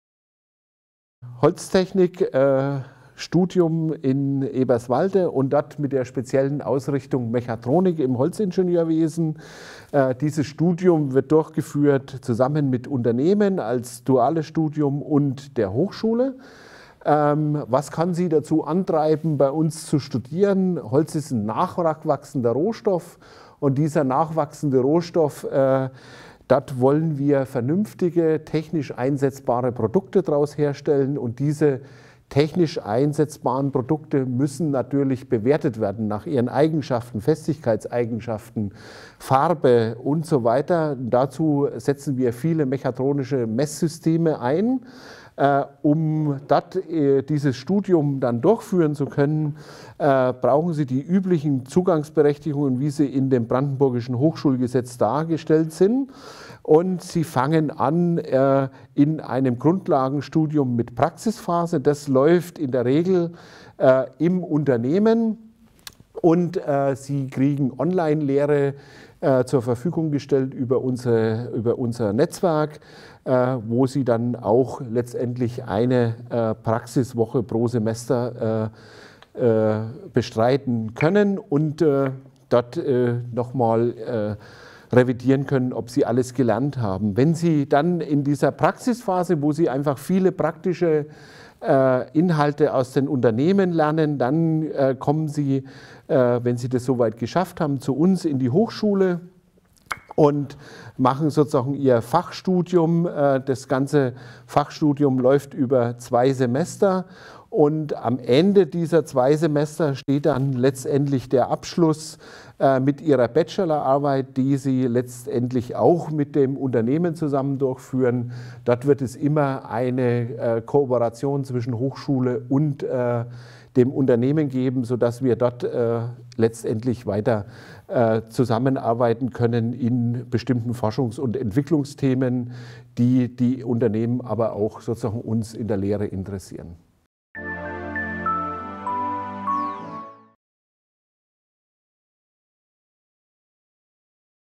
"Mechatronik im Holzingenieurswesen" - Unser dualer Bachelorstudiengang stellt sich vor
Du interessierst dich für den Studiengang "Mechatronik im Holzingenieurswesen"? Ein duales Studium ist vielleicht genau das Richtige für dich? Unsere Studiengangsleiter*innen stellen dir den Studiengang und seine Besonderheiten hier vor! Sei gespannt, was unser einzigartiges Studienangebot an der Hochschule für nachhaltige Entwicklung in Eberswalde für dich bereit hält!